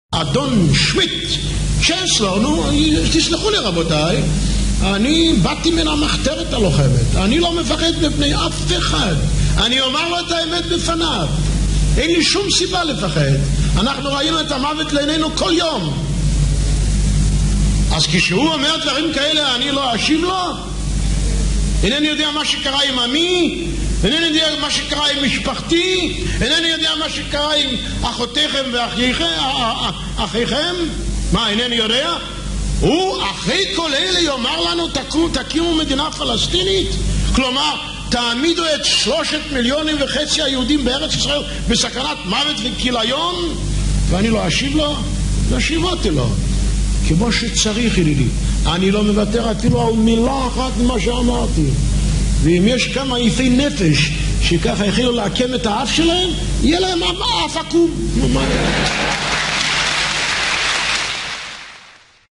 מנחם בגין עונה בחריפות לקנצלר גרמניה 1981
הקנצלר האשים את ישראל בחוסר מוסריות, ובגין תקף, כשהשמאל דרש מבגין להתנצל, בגין ענה בנאום מרתק